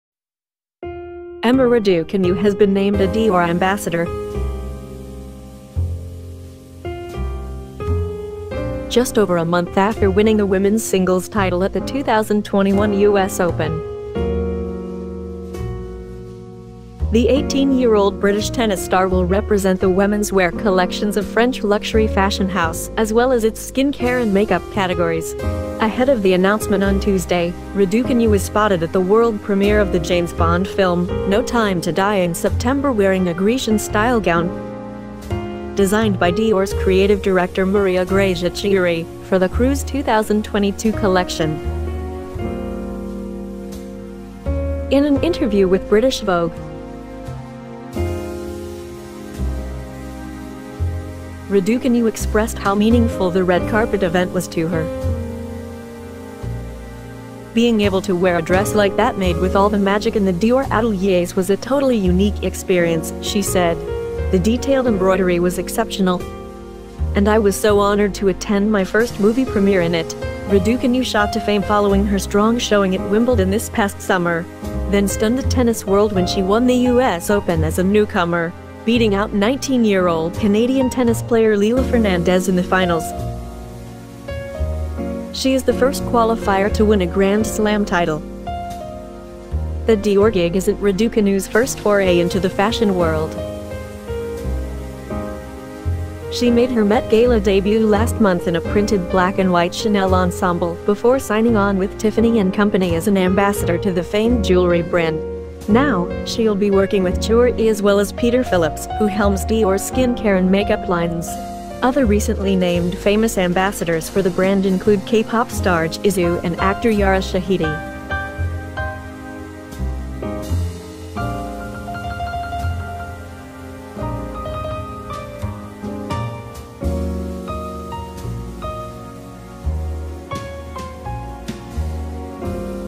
Emma Raducanu is the new face of Dior
EmmaRaducanu is very excited to join this iconic house, thank you @Dior💖